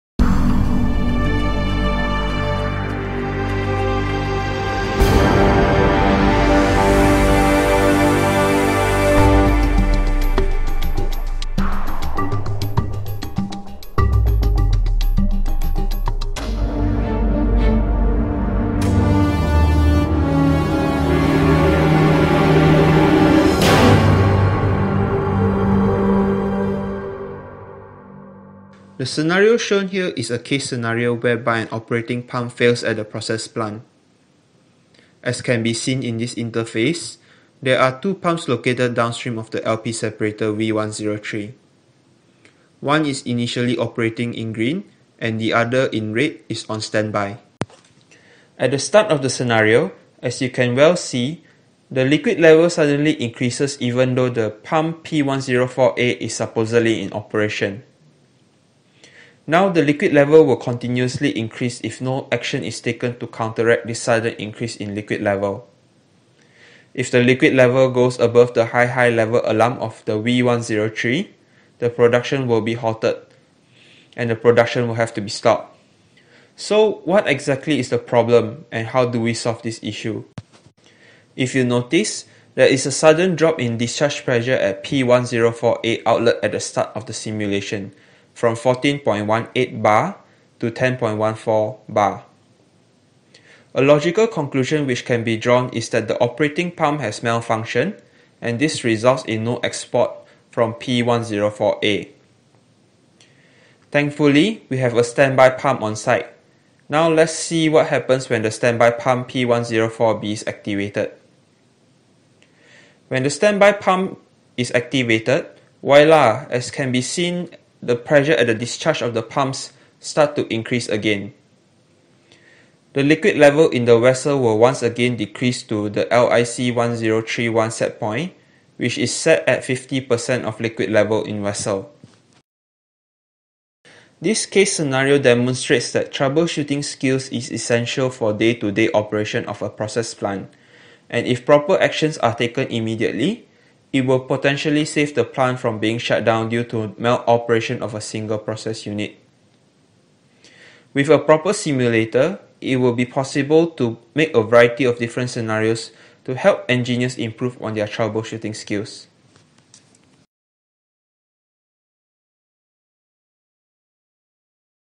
Interactive Learning Simulation Tool for Process Engineers (Pump Scenario)
Example of using Synergy's process simulation tool for operator training.